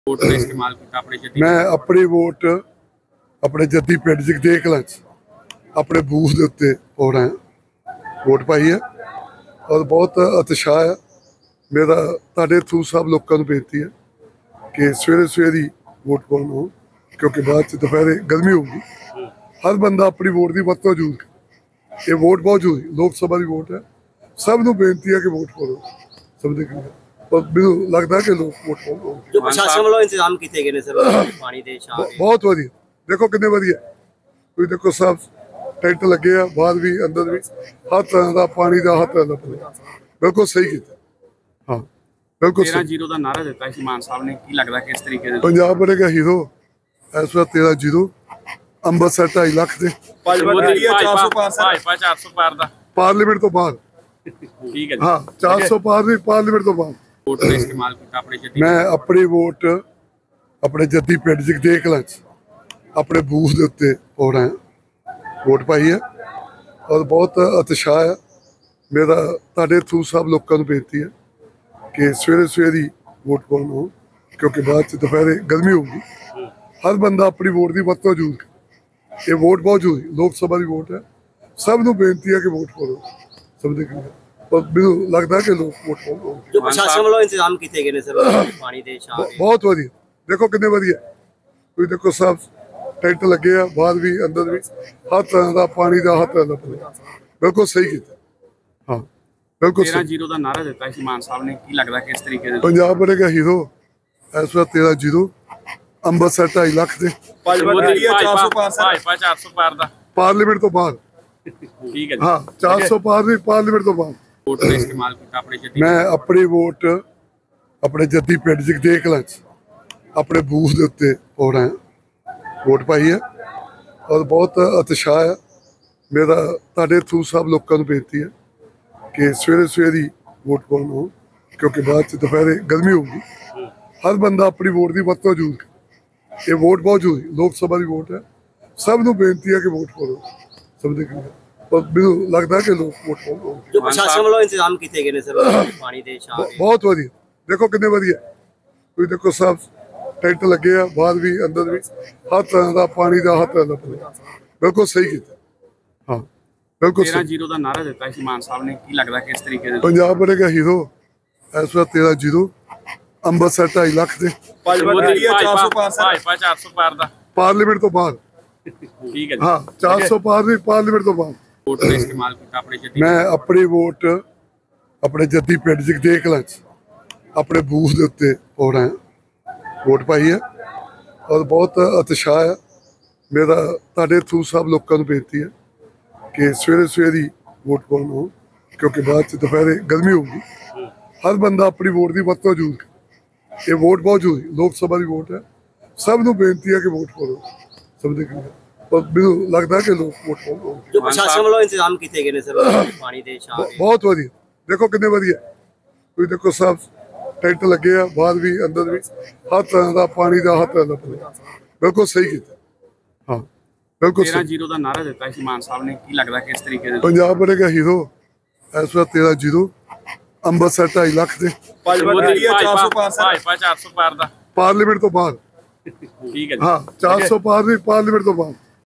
Kuldeep Dhaliwal ਨੇ ਵੋਟ ਪਾ ਕੇ ਦੁਹਰਾਇਆ 13-0 ਦਾ ਨਾਅਰਾ | Lok Sabha 7th Phase Polling | Amritsar Seat
ਕੁਲਦੀਪ ਧਾਲੀਵਾਲ ਨੇ ਵੋਟ ਪਾ ਕੇ ਦੁਹਰਾਇਆ 13-0 ਦਾ ਨਾਅਰਾ | Lok Sabha 7th Phase Polling | Amritsar Seat